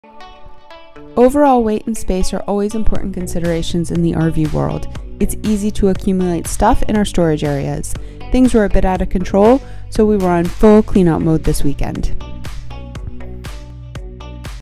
Cleaning Out the Storage Area - RV Weight and Space Issues
SPRING CLEANING 💦 🧽

Our storage area was a little junked up with stuff... so it was time to declutter and reorganize!

➡️ Space & Weight are HUGE with the RV!

⁉️ Do you keep your storage organized?!?

~ Sarah & Matt